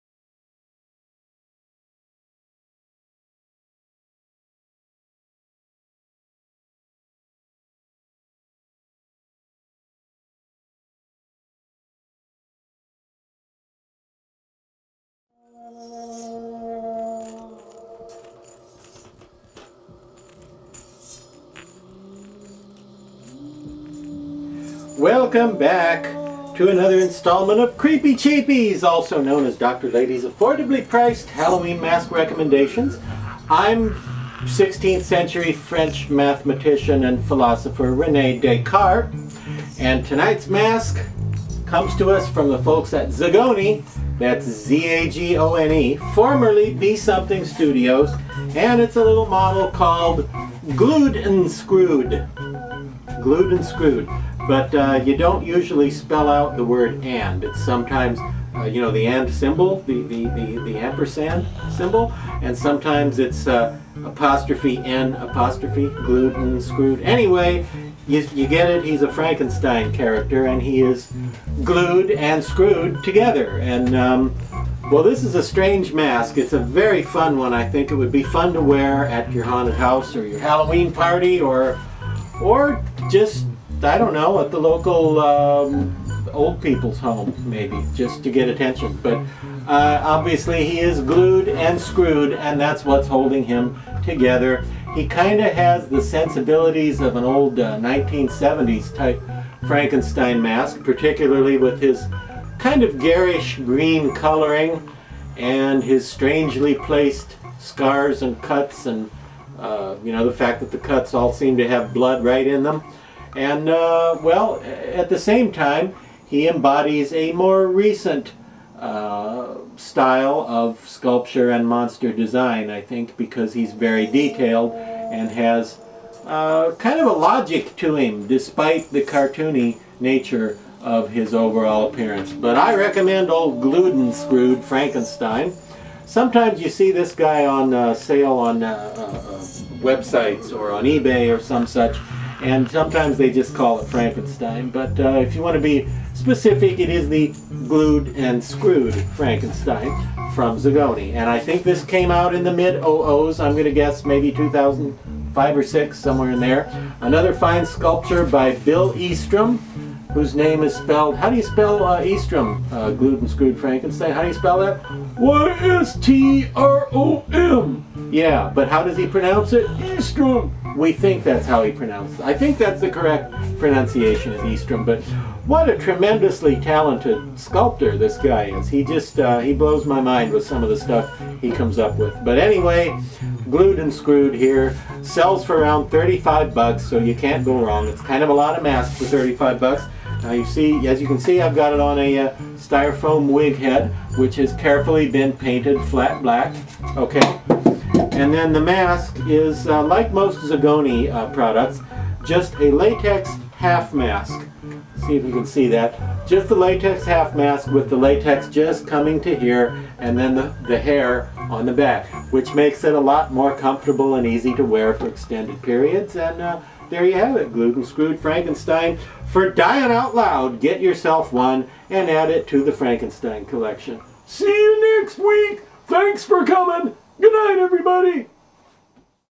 Creepy Cheapies: Glued & Screwed
DR LADY discusses a cost-effective Frankenstein.